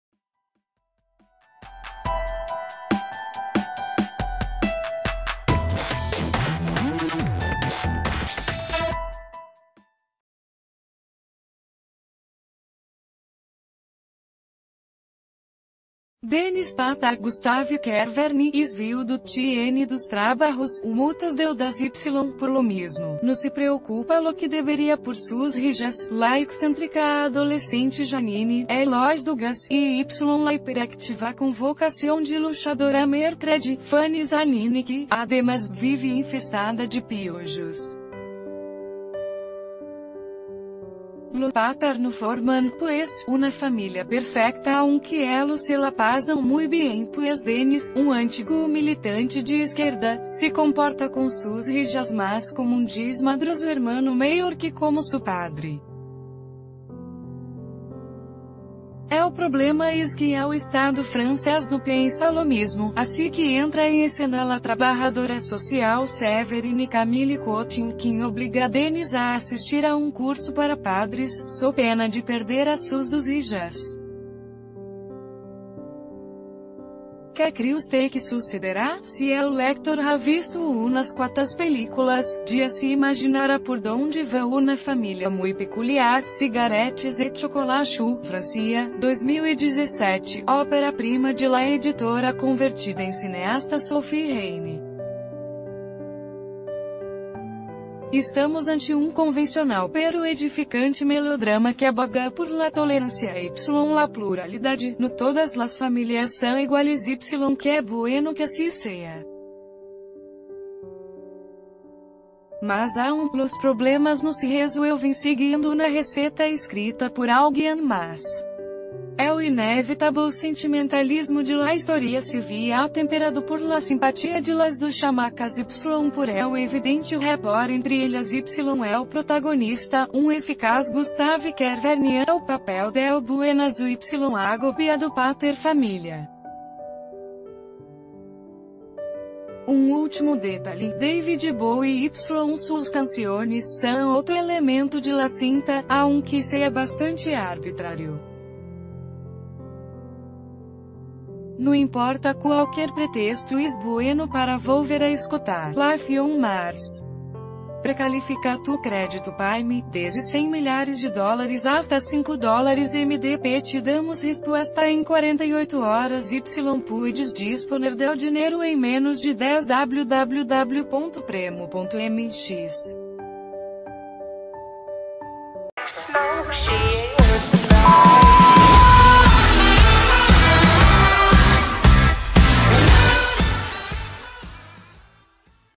La película Una Familia muy Peculiar se estrena en la Sala de Arte de Cinépolis
Denis Patar (Gustave Kervern) es viudo, tiene dos trabajos, muchas deudas y, por lo mismo, no se preocupa lo que debería por sus hijas, la excéntrica adolescente Janine (Héloïse Dugas) y la hiperactiva con vocación de luchadora Mercredi (Fanie Zanini) que, además, vive infestada de piojos. Los Patar...